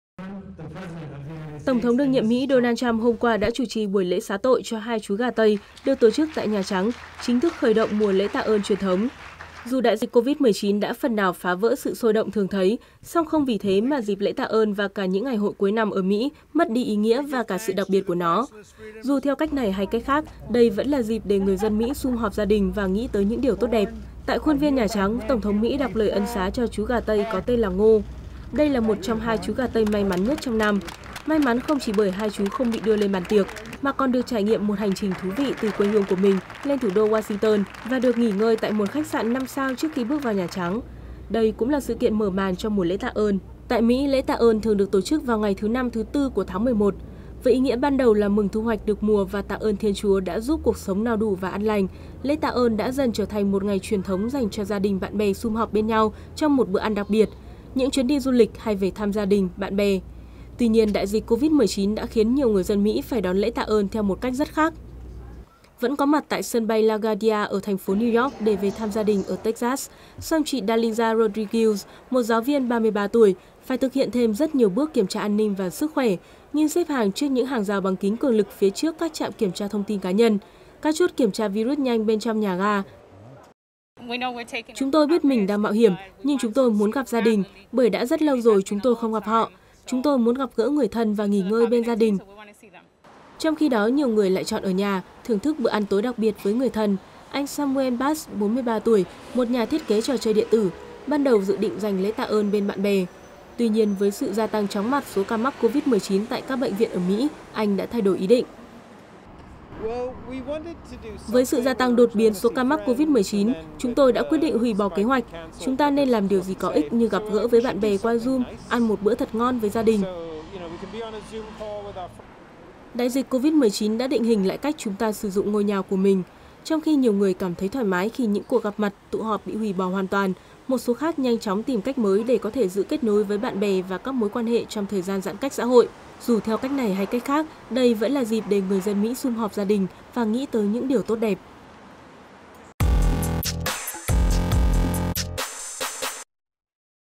Mỹ bước vào mùa Lễ Tạ ơn đặc biệt nhất
Tổng thống đương nhiệm Mỹ Donald Trump hôm qua đã chủ trì buổi lễ xá tội cho 2 chú gà tây được tổ chức tại Nhà trắng, chính thức khởi động mùa lễ Tạ ơn truyền thống. Dù đại dịch Covid-19 đã phần nào phá vỡ sự sôi động thường thấy, song không vì thế mà dịp Lễ Tạ ơn và cả những ngày hội cuối năm ở Mỹ mất đi ý nghĩa và cả sự đặc biệt của nó. Dù theo cách này hay cách khác, đây vẫn là dịp để người dân Mỹ sum họp gia đình và nghĩ tới những điều tốt đẹp.